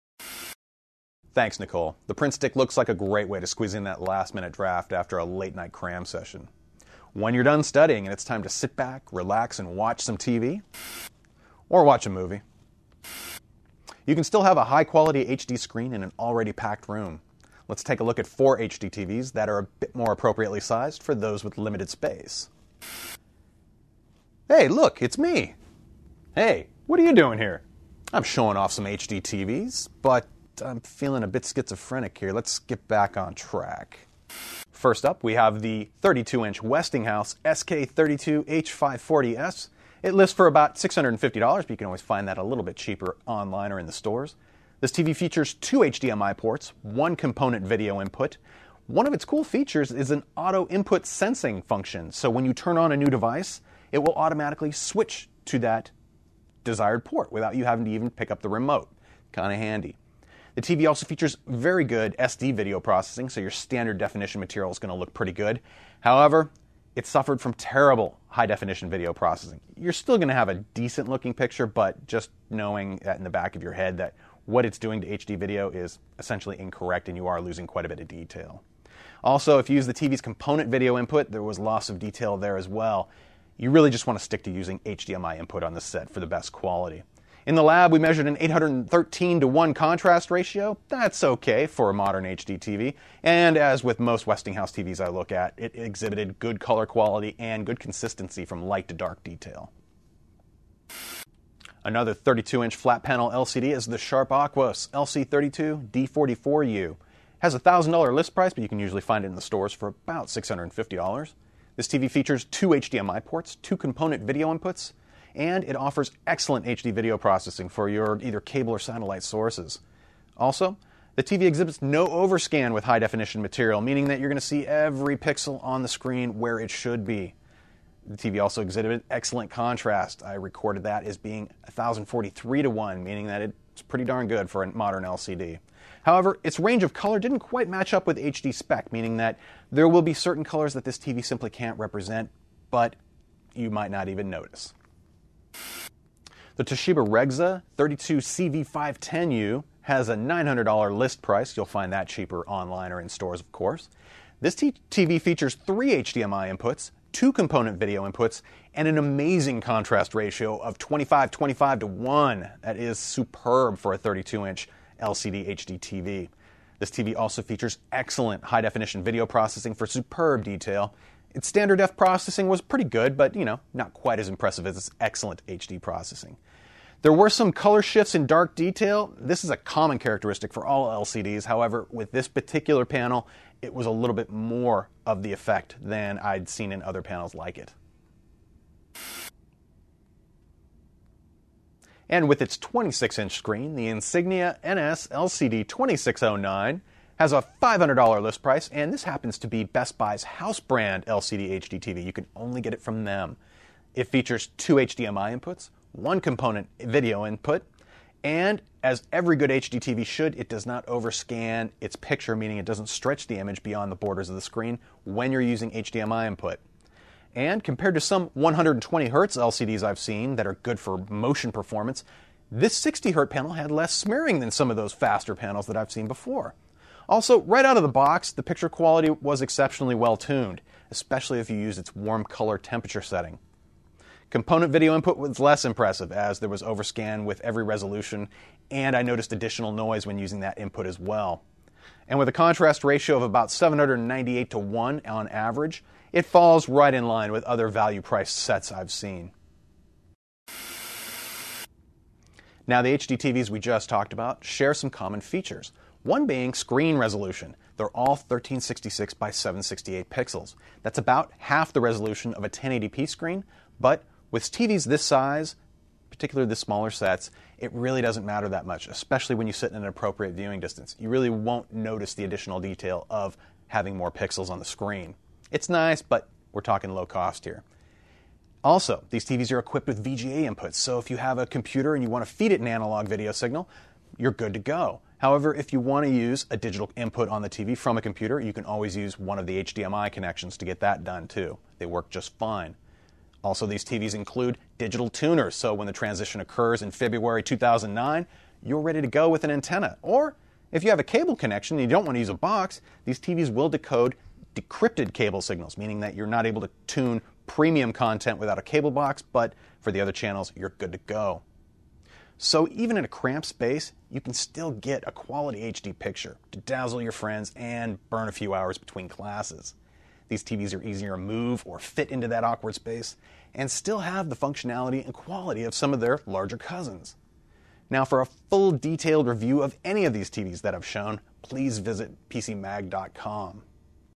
Small Screen HDTVs
Robert has 4 small screen HDTVs that will be great for the dorm room.